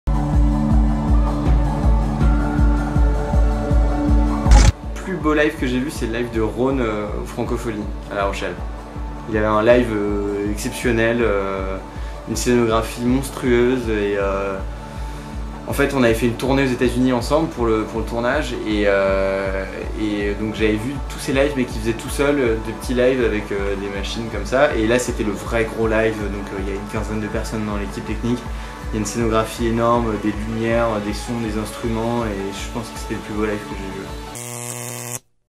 Julian Starke - French Waves : le plus beau live électro ?
Interview de Julian Starke, réalisateur  du documentaire et de la web série French Waves sur la musique électronique française.
Il raconte son plus beau live électro vu : Rone aux Francofolies de la Rochelle. Exceptionnel, scénographie monstrueuse avec une équipe technique de 15 personnes...